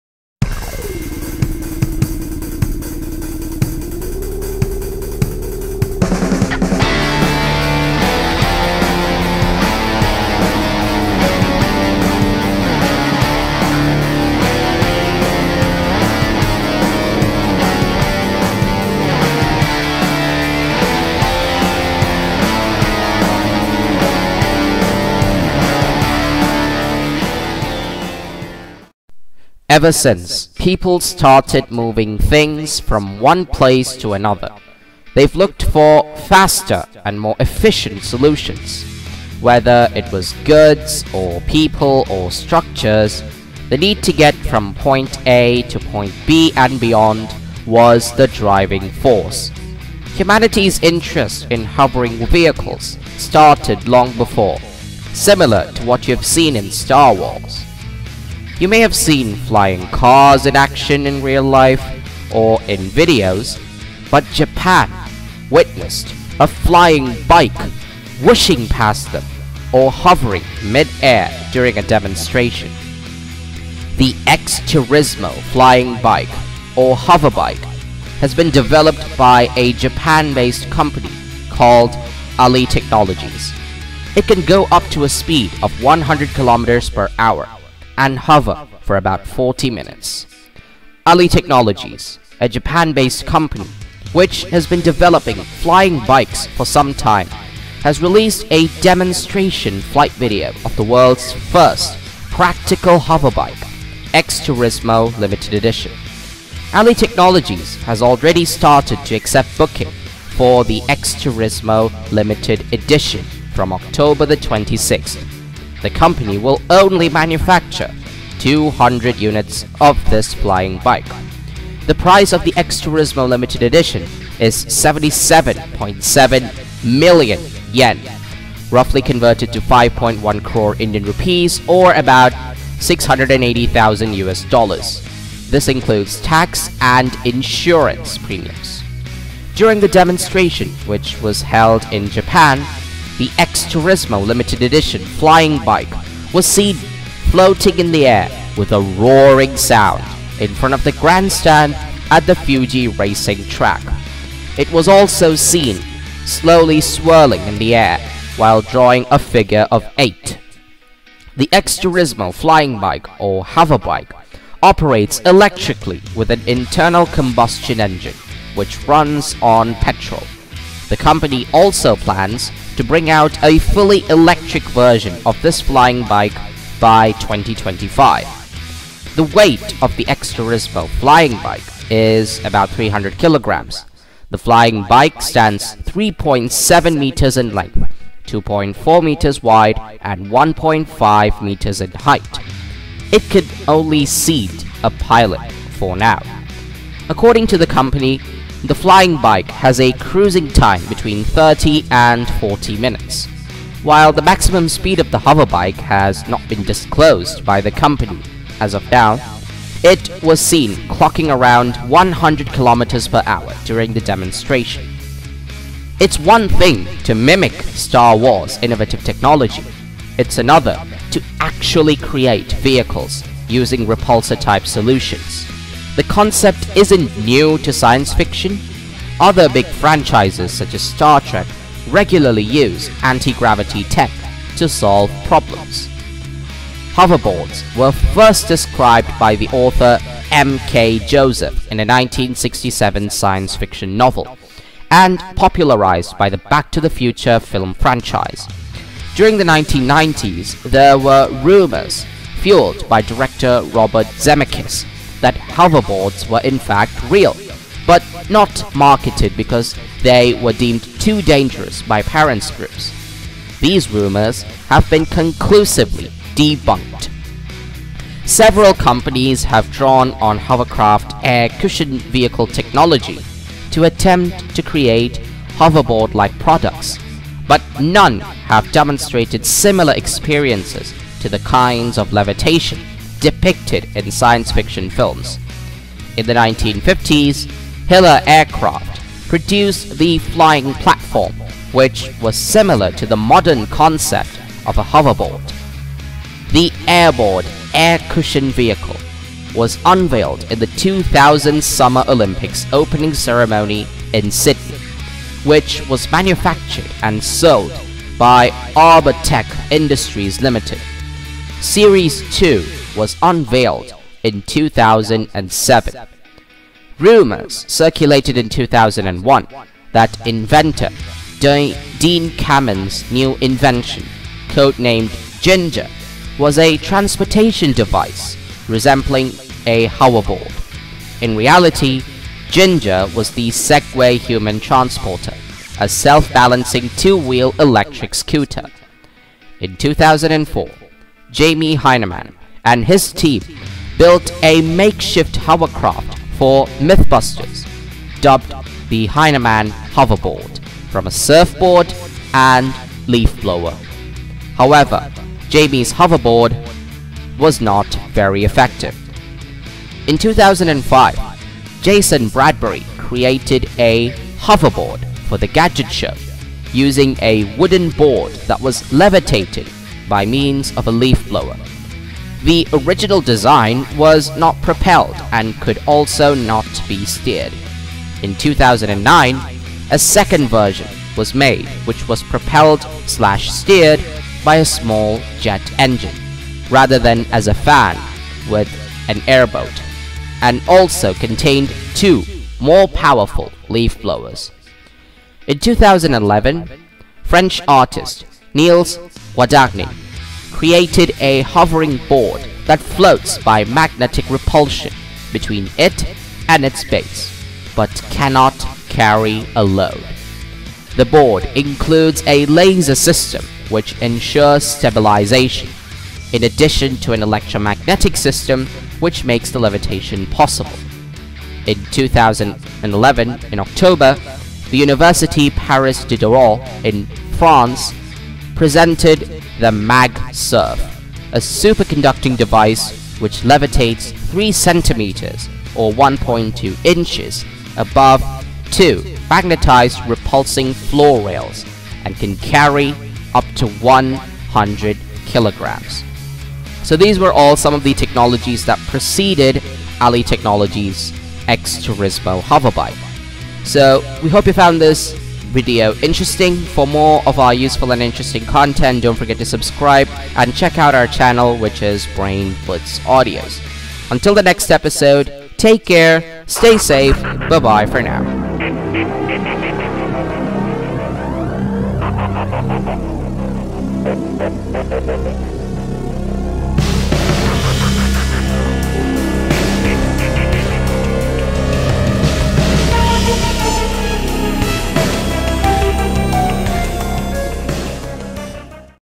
World's First Flying Bike/XTURISMO/Hoverbike Takes Flight | Japan | US Debut | Star Wars Flying Bike
XTURISMO Limited Edition. ALI Technologies, a Japan-based company which has been developing flying bikes for some time, has released a demonstration flight video of the world's first practical hover bike.

00:00 A.L.I Technologies
01:00 XTURISMO Japan Unveils Flying Bike
02:00 Price of XTURISMO
03:00 Electric Version by 2025
04:00 M.K Joseph Science Fiction Novel
05:00 Flying Platform: Air board Air Cushion Vehicle
06:00 MythBusters
07:00 The Mag surf